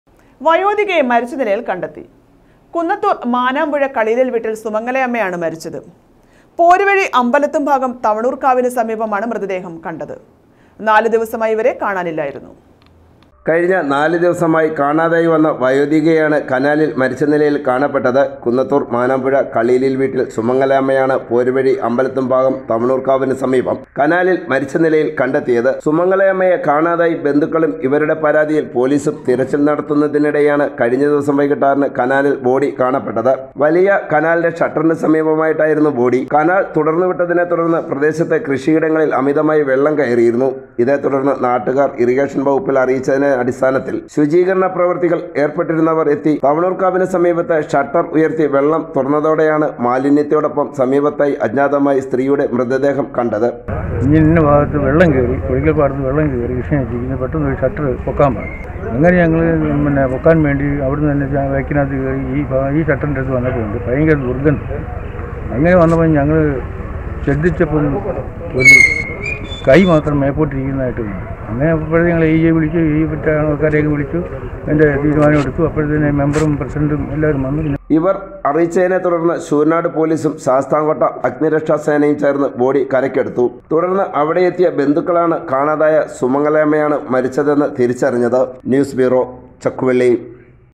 വയോധികയെ മരിച്ച നിലയില്‍ കണ്ടെത്തി....
വയോധികയെ മരിച്ച നിലയില്‍ കണ്ടെത്തി. കുന്നത്തൂര്‍ മാനാമ്പുഴ കളീലില്‍വീട്ടില്‍ സുമംഗലയമ്മയാണ് മരിച്ചത് .പോരുവഴി അമ്പലത്തും ഭാഗം തവണൂര്‍ക്കാവിന് സമീപമാണ് മൃതദേഹം കണ്ടത്. നാലു ദിവസമായി ഇവരെ കാണാനില്ലായിരുന്നു.